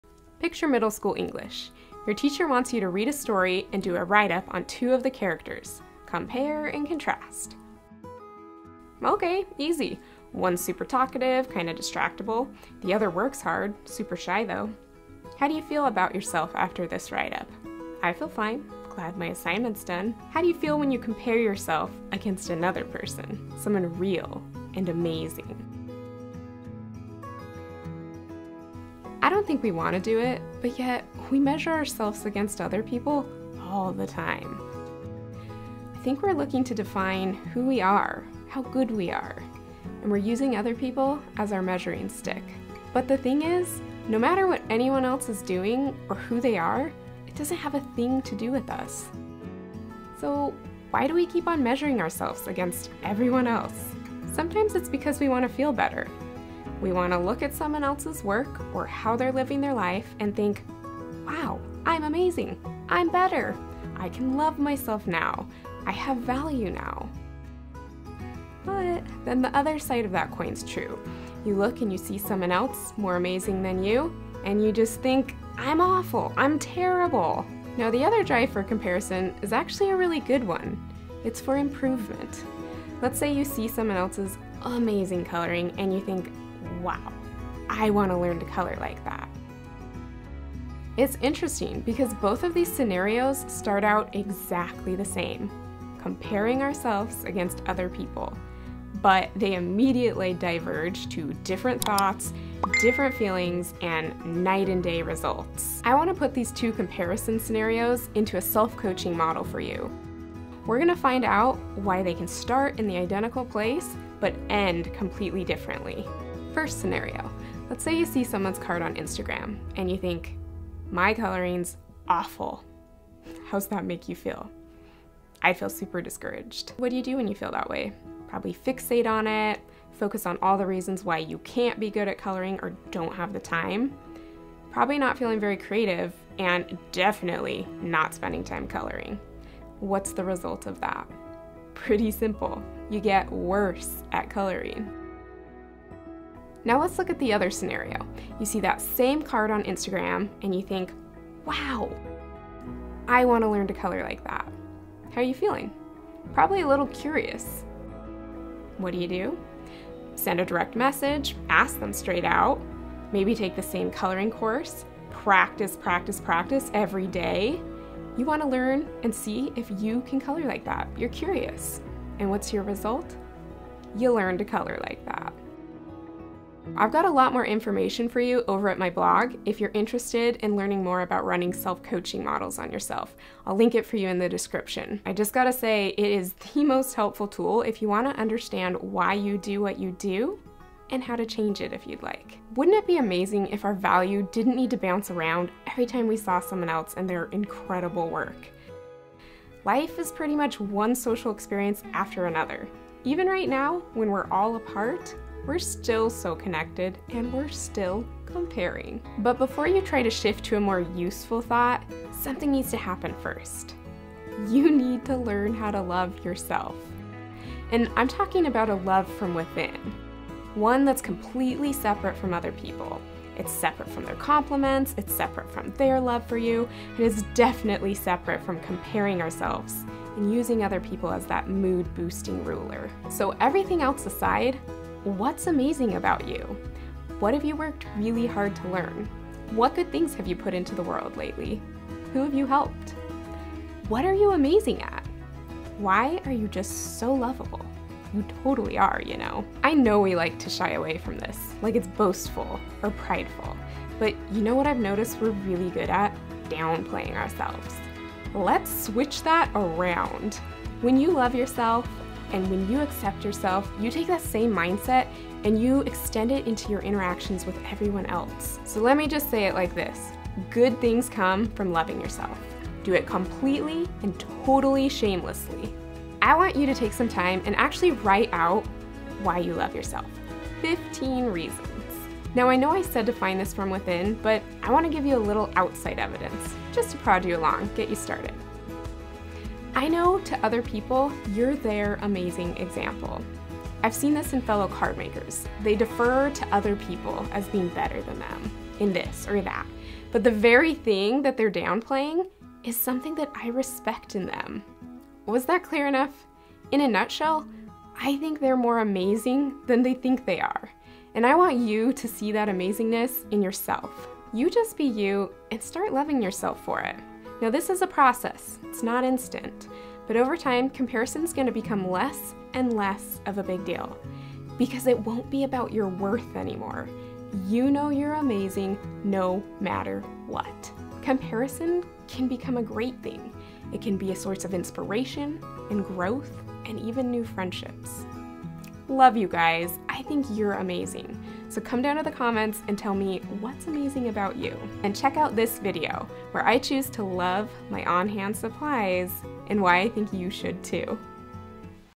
CREATIVE COMPARISON a Good Thing?
Escape the social media comparison trap with a little self love for your creative side. You're not the only maker battling creative self doubt. I'm sharing two ways we experience comparison: one very damaging to and the other massively helpful for our creative growth. Check it out and replace self doubt with lots of self love for your creative side.

M A K E M O R E T O G E T H E R H E R E